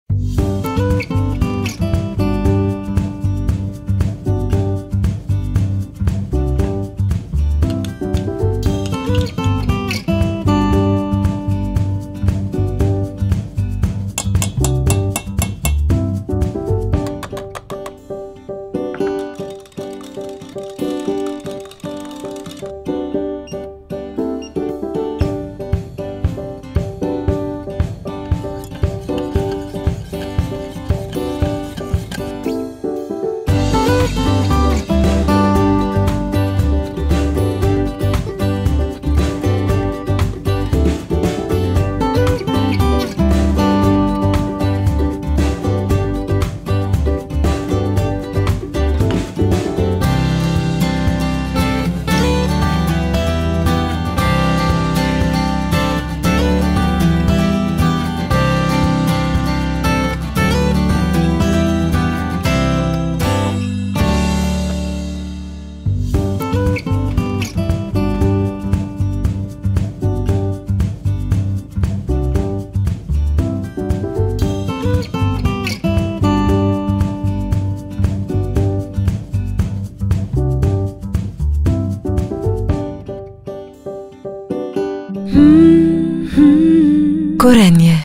HomeMade recipe: Vegan fried egg • Simple Meals by Gorenje
Vegan fried egg

Serves: 3 eggs

Ingredients for egg whites:

● 25 g rice flour
● 1 tsp cornstarch
● 120 ml rice milk

Method:

1. Add 70 g pumpkin puree to a small pot.
2. Add ¼ tsp salt, ¼ tsp mustard, ⅛ tsp turmeric and ⅛ tsp nutritional yeast.
3. Mix 4 tbsp water and 1tsp cornstarch separately.
4. Pour the cornstarch mixture to the pumpkin puree.
5. Mix everything and heat it up until it starts boiling.
6. Set aside.
7. In a small bowl add 25 g rice flour, 1 tsp cornstarch, ⅛ tsp salt and ⅛ tsp garlic powder.
8. Mix.
9. Pour in 120 ml rice milk and mix.
10. Coat a non-stick pan with oil and heat it up on a low heat.
11. Pour in 2 tbsp of egg whites.
12. Cook them for 1-2 minutes, then add 1 tbsp of egg yolk in the middle.
13. Cover the pan with a lid or a plate.
14. Cook for 2-3 minute on low heat.
15. Serve eggs on a toast with avocado spread.